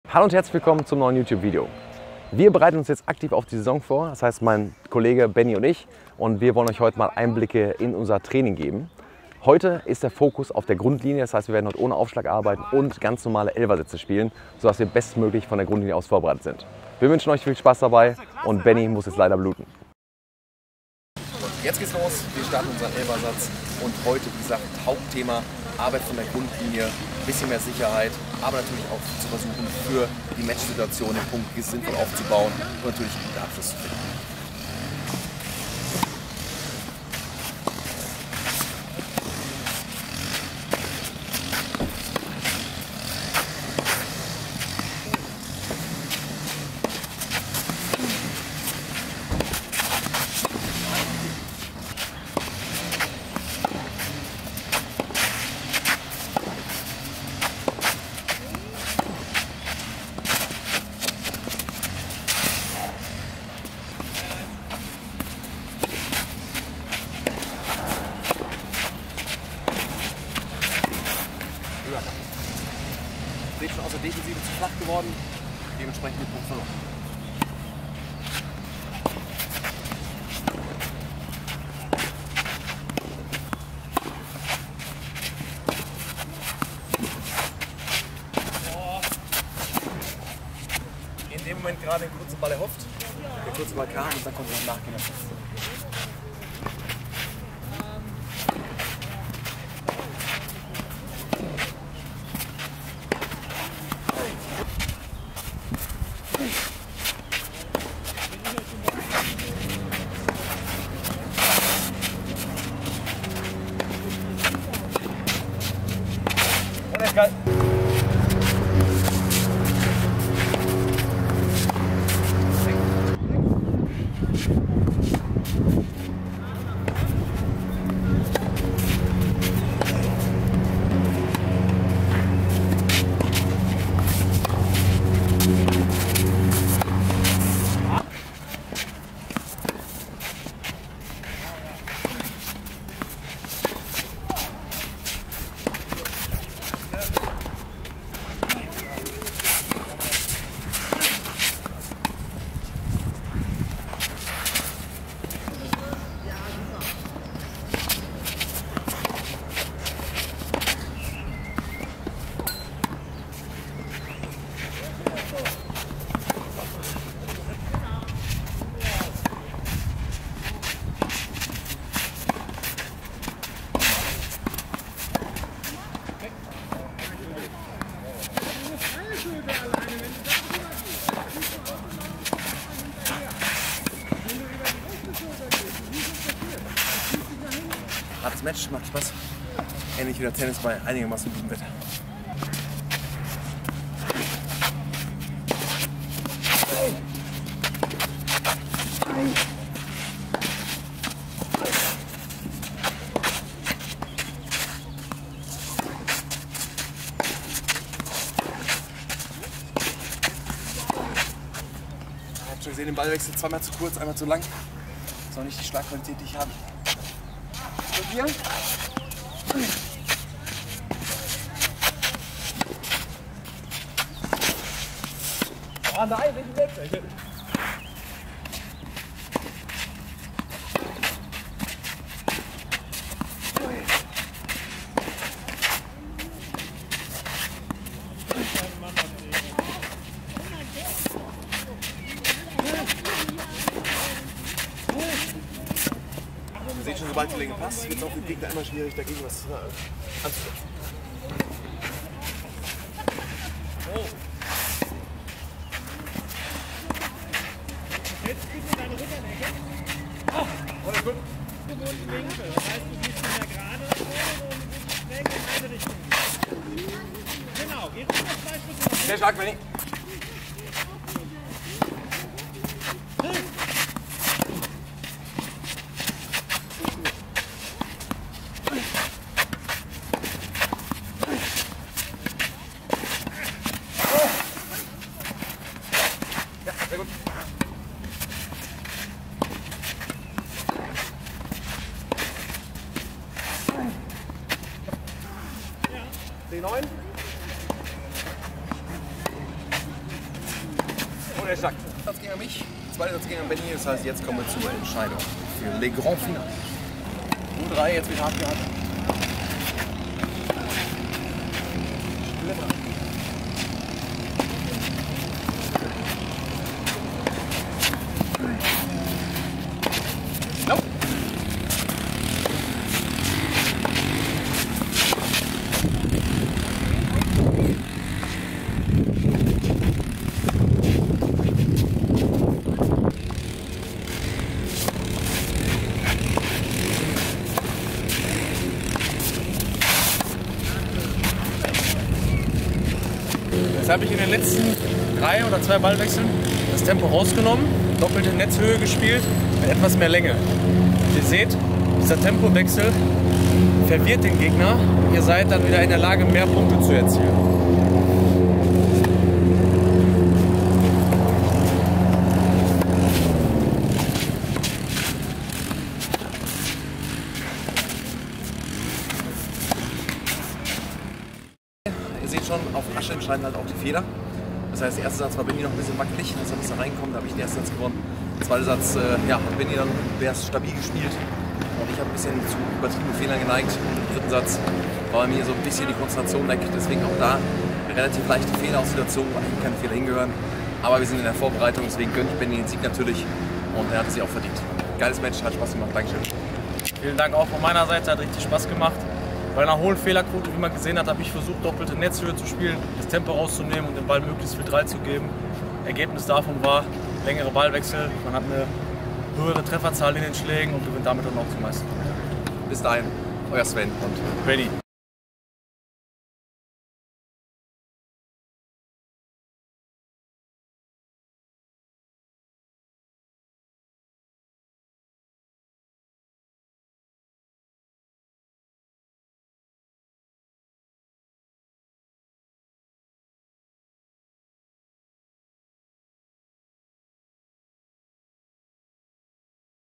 Trainings-Match! Sven vs. Benni
In dem heutigen Video spielen Benjamin Fitzon und ich ein Trainings-Match. Wir bereiten uns für die kommende Saison vor und der Fokus liegt deshalb auf der Grundlinie.

Ich hoffe, dir gefällt unser Tennis Match. Ich freue mich schon sehr auf dein Feedback. 😊

Und wenn du das volle Potenzial aus dir und deinem Tennisspiel herausholen willst, dann melde dich hier für meine Tennis Masterclass an